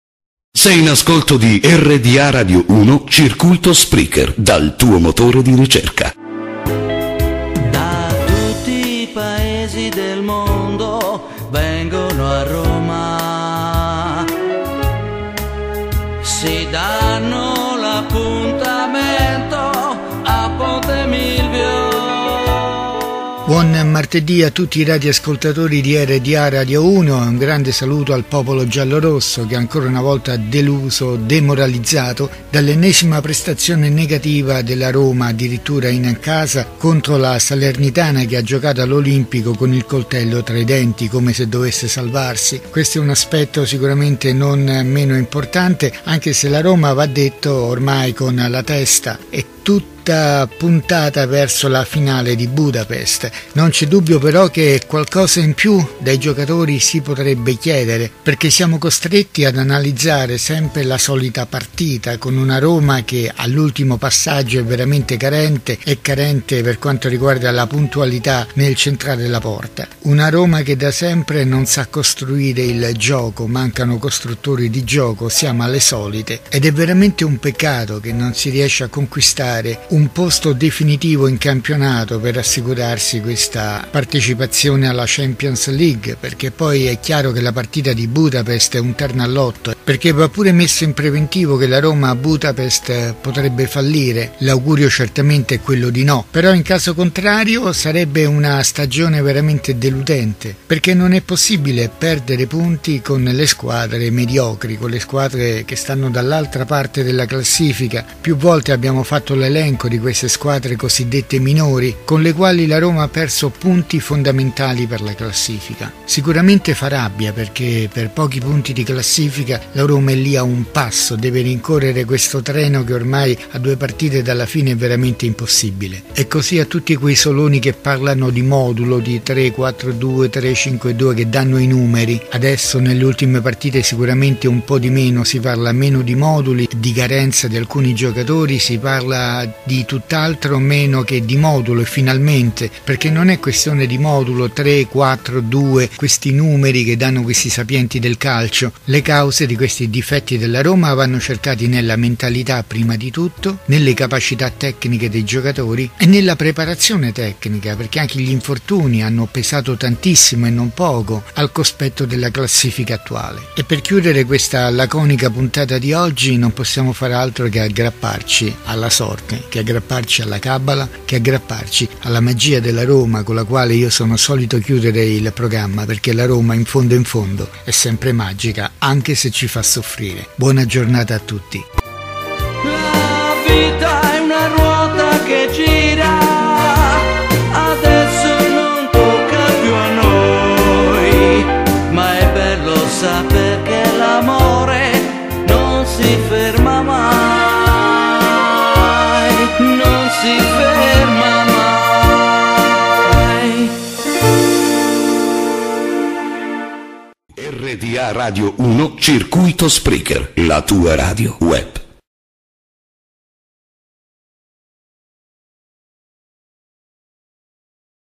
Roma Salernitana 2-2 di Tony Mantineo
Analisi della partita Roma Salernitana per il 36° turno di Campionato italiano di Calcio Divisione Serie A Stagione 2022-23.  A cura del telecronista Tony Mantineo